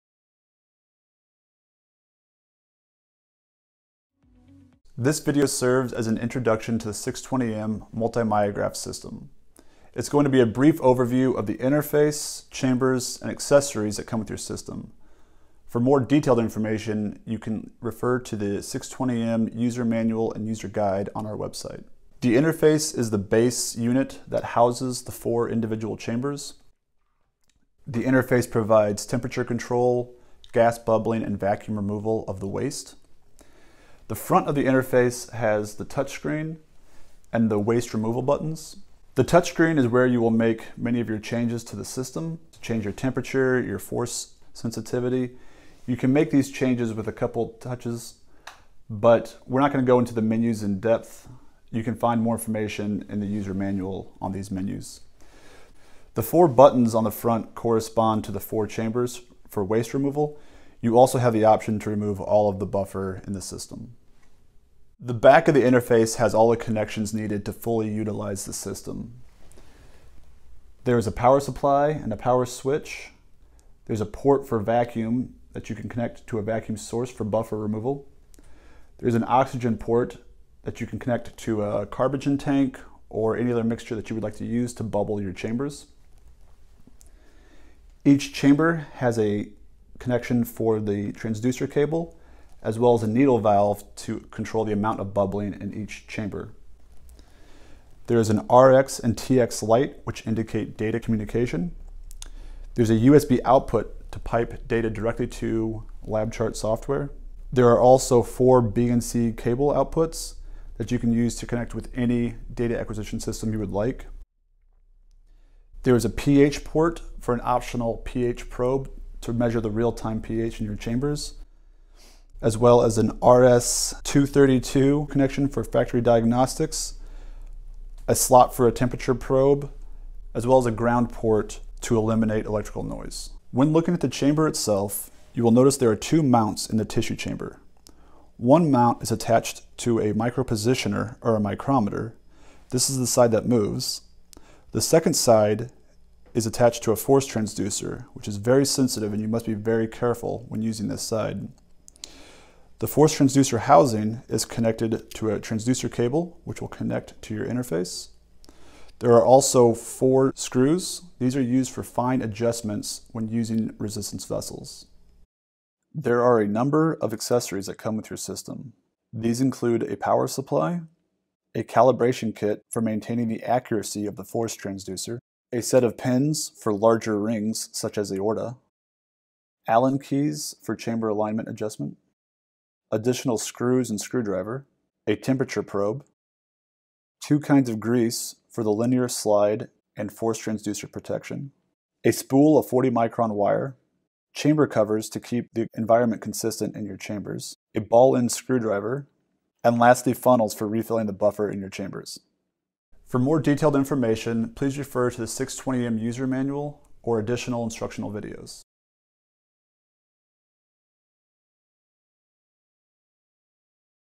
System introduction – DMT Multi Myograph 620M
A brief overview of the interface, chambers and accessories that are supplied with your system.